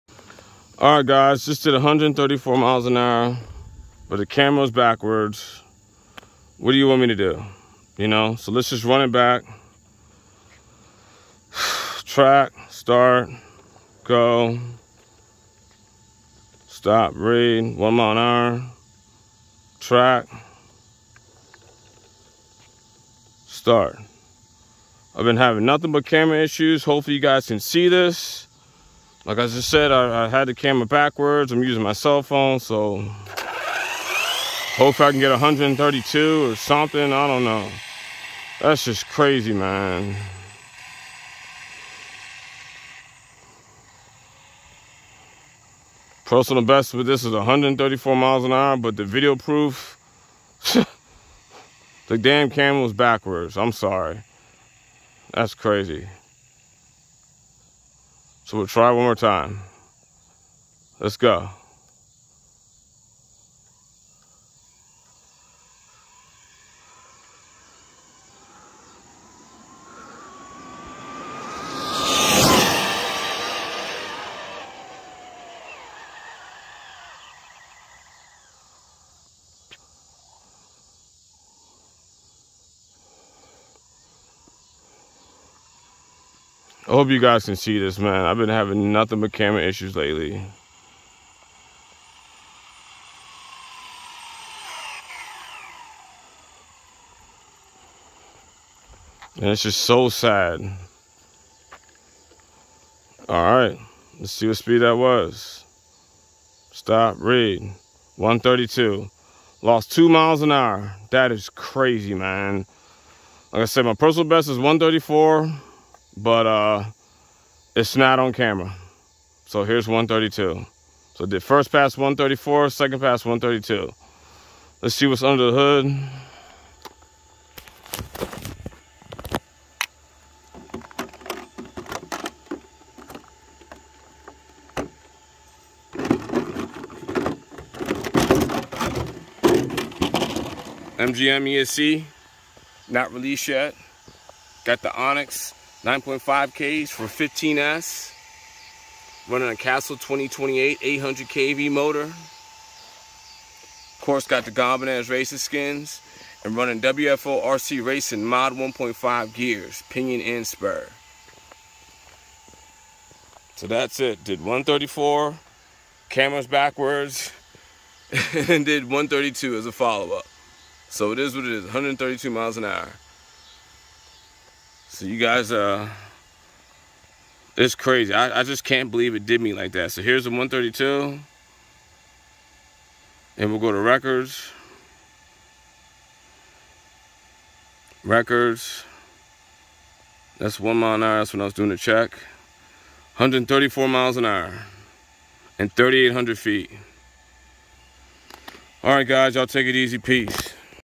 132 MPH Arrma Kraton [MGM-CASTLE-ONYX]
Back at it again and a new 132 recorded PB and a new 134 un recorded PB.   And this Arrma Kraton is putting down some serious power with this unreleased MGM 15s esc and just a standard old castle 2028 800kv motor.  Not to mention this ONYX 9500 HV pack are hitting super hard, and I am only charging to 4.20 volts, these lipos handle 4.35 volts with ease. The WFO mod 1.5 gears are holding down some serious power.  And of course the gone bananas racing skin foam wheels are always doing their job.  I have been working pretty close with MGM with firmware's for this esc.  This last firmware seems almost infinite with the power, but at the same time the esc wont fail because the way the MGM developers allow the power while guard guarding critical hardware component's with the software.  MGM is definitely on to something here, I expect them to be dropping some serious heat soon.